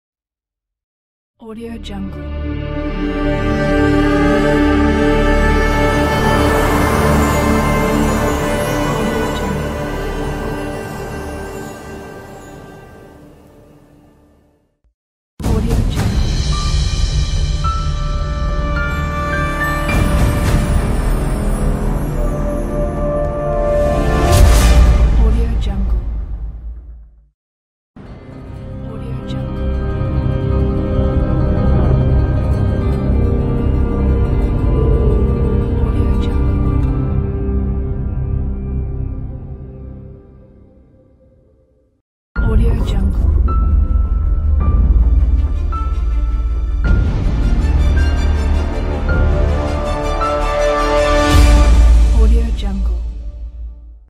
Sand Logo Opener
Very realistic opener, where logo appears from the sand. Project works with text and graphic. Universal opener and can be used for adventures of any kind from Indiana Jones until post-apocalyptic.